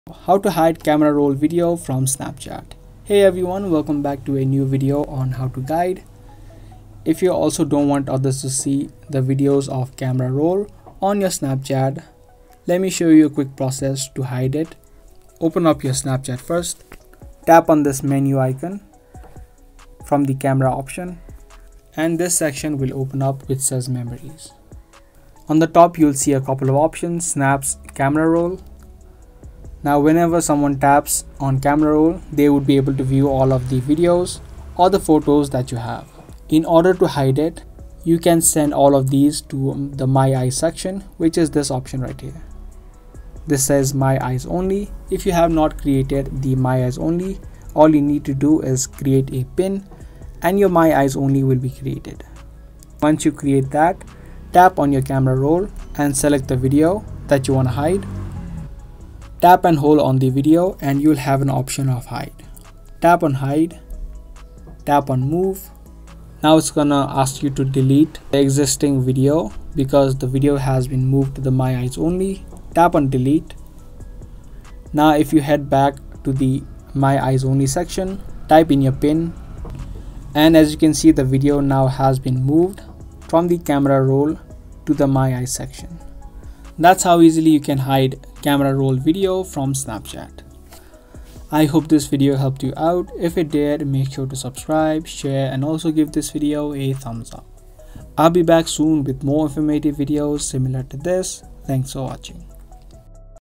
How To Hide Camera Roll Video In Snapchat (Quick & Easy)
Learn how to hide camera roll videos on snapchat. Protect your personal videos by hiding camera roll videos from snapchat.

Do you wish to hide your videos from snapchat and keep it private only for you to see? Are you confused how to do so? In this video, we'll show you how to hide camera roll videos from Snapchat.

If you're worried about your friends or followers seeing personal videos from your camera roll, this video is for you. We'll guide you through the process step-by-step and show you how to keep your private videos safe from prying eyes. By the end of this video, you'll have the knowledge and tools to protect your privacy on Snapchat. Don't miss out on these valuable Snapchat privacy tips!